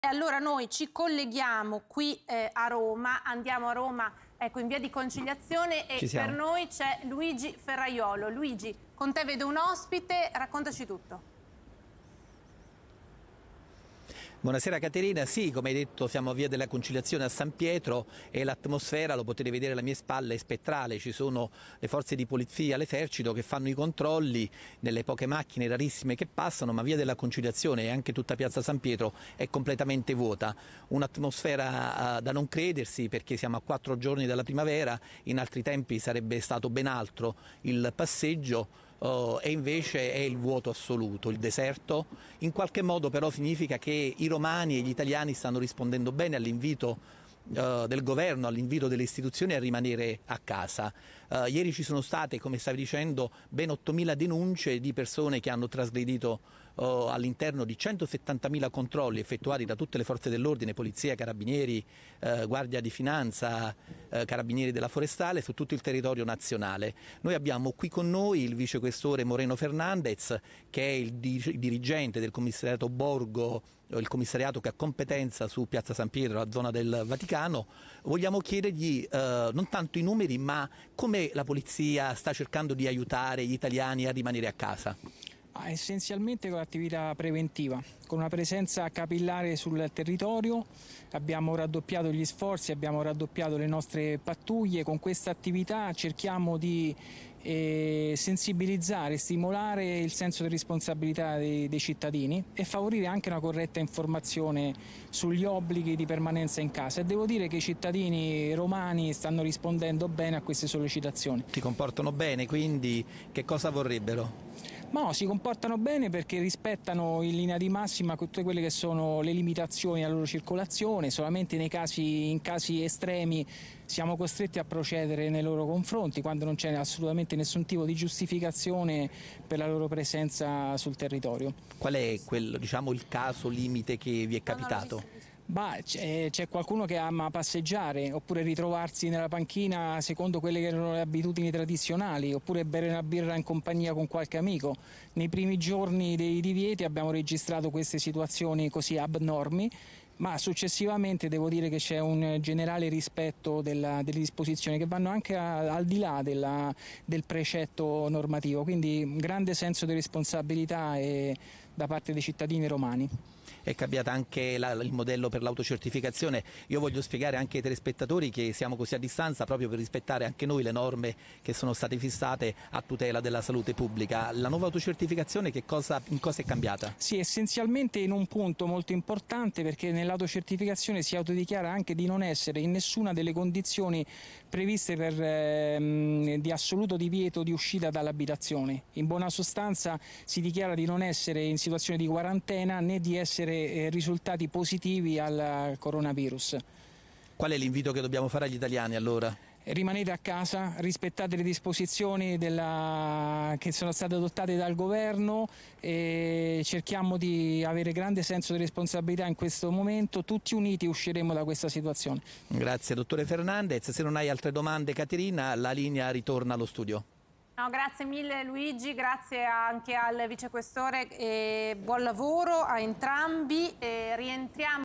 San Pietro deserta, ma l'Italia risponde bene al coronavirus: 8mila denunciati in un giorno
Il collegamento in diretta del giornalista Luigi Ferraiuolo con il Vicequestore di Roma Moreno Fernandez, da Via della Conciliazione, poco prima di Piazza San Pietro, per raccontare per Tgtg di Tv2000, come la Polizia e tutte le forze dell'ordine stanno aiutando gli italiani a combattere il Covid-19 e a restare in casa